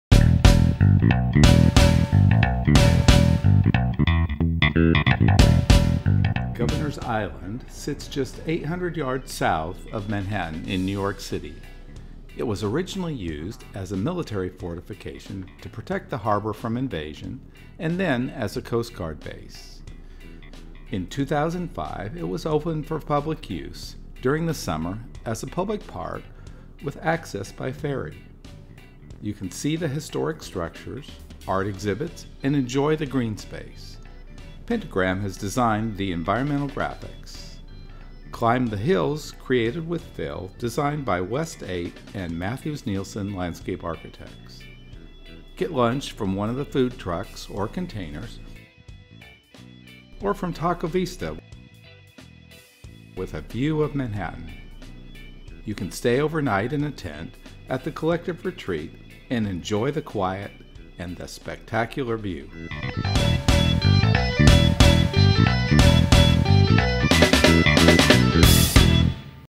Governors Island NYC - MNLA & West 8
Governors Island sits just 800 yards south of Manhattan in New York City. It was originally used as a military fortification to protect the harbor from invasion and then a Coast Guard base.

In 2005 it was opened for public use, open during the summer as a public park with access by ferry. You can see the historic structures, art exhibits and enjoy the green space.

Pentagram has designed the environmental graphics. Climb “The Hills” created with fill that were designed by West 8 and Mathews Nielsen Landscape Architects.

Get lunch from one of the food trucks or containers or from Taco Vista with a view of Manhattan. You can stay overnight in a tent at the Collective Retreat and enjoy the quiet and spectacular view.

Collective Retreats Governors Island website

Taco Vista

West 8

Mathews Nielsen Landscape Architects

Pentagram